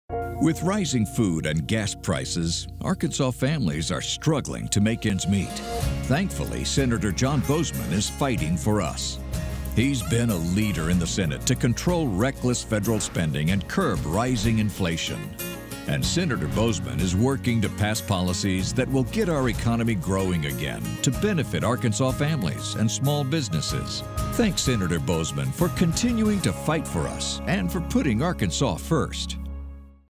Highlighting Sen. John Boozman
Sen. John Boozman is a leader in the Senate to control reckless federal spending and curb rising inflation.